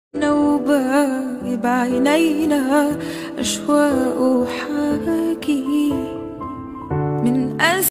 spotlight Eye makeup 💥🔥💯🤩
it very easy to create spot eye makeup
many of.you have been asking this is how to spotlight eye makeup
halo cut Crease eye makeup